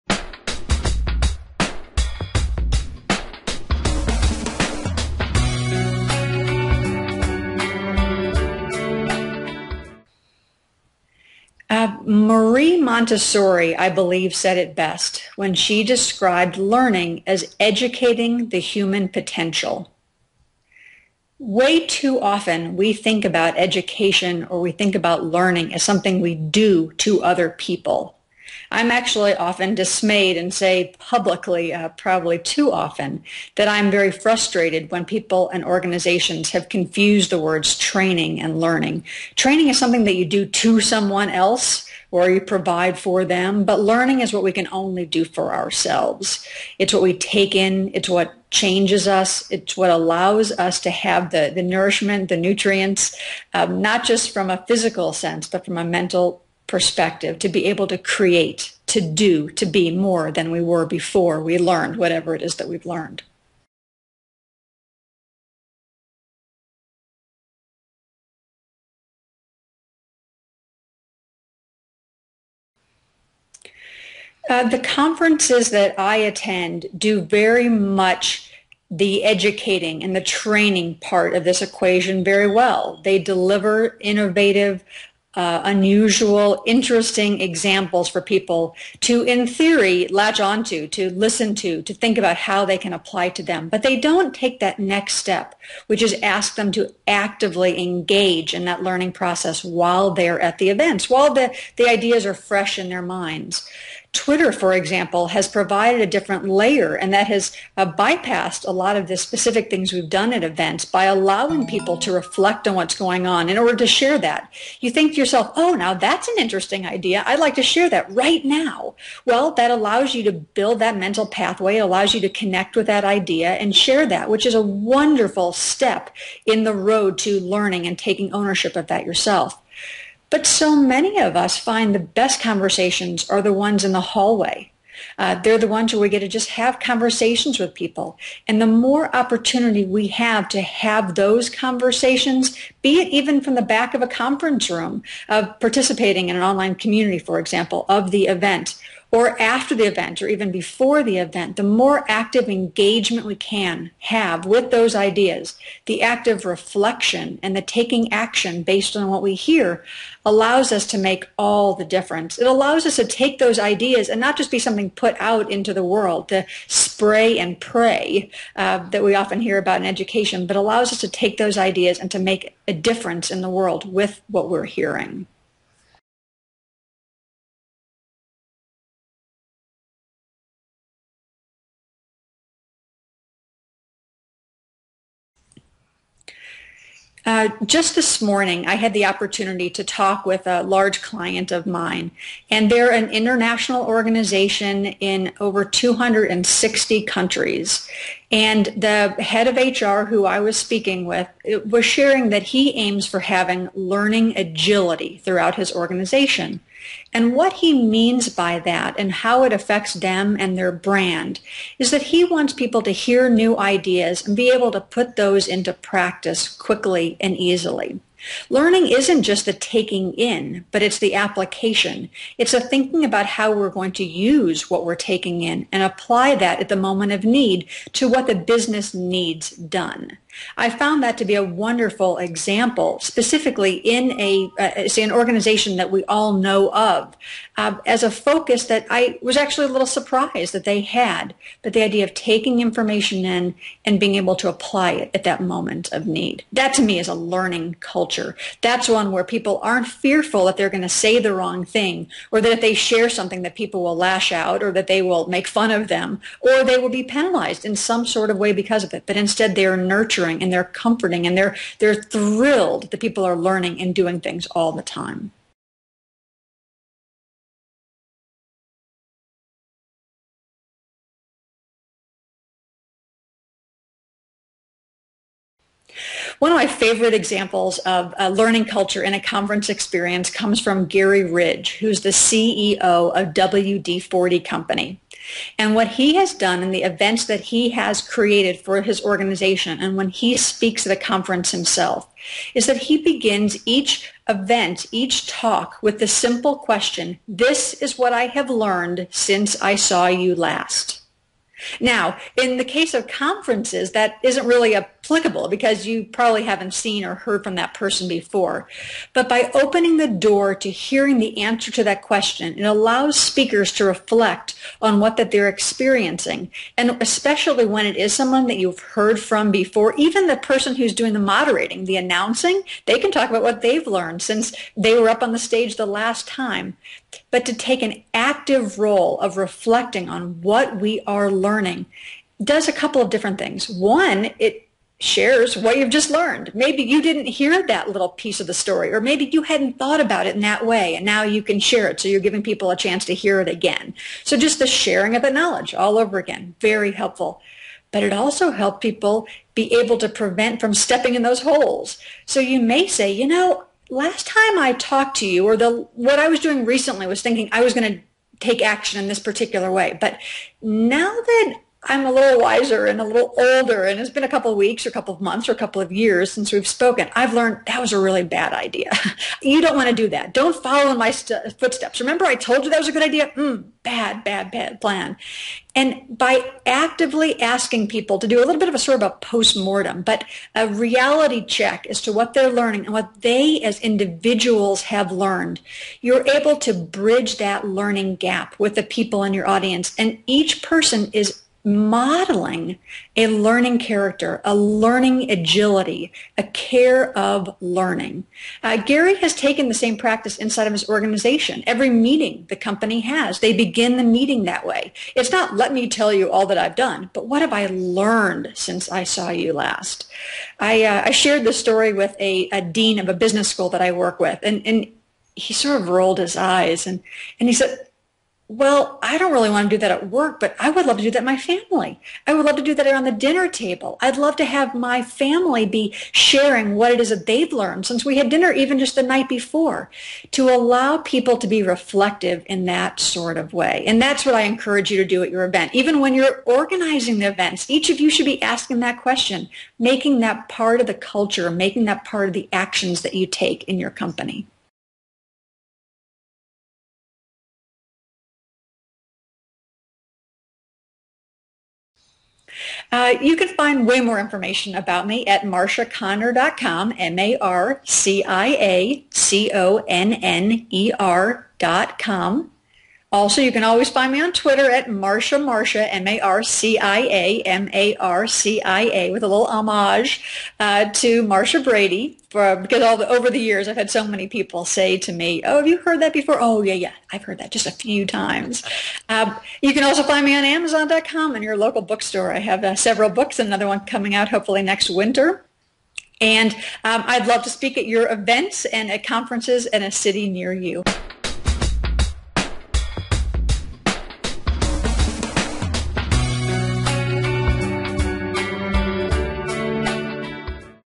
Creating Conference Cultures Of Learning
Author, blogger, learning expert and speaker Marcia Conner shares her thoughts about conference education and learning.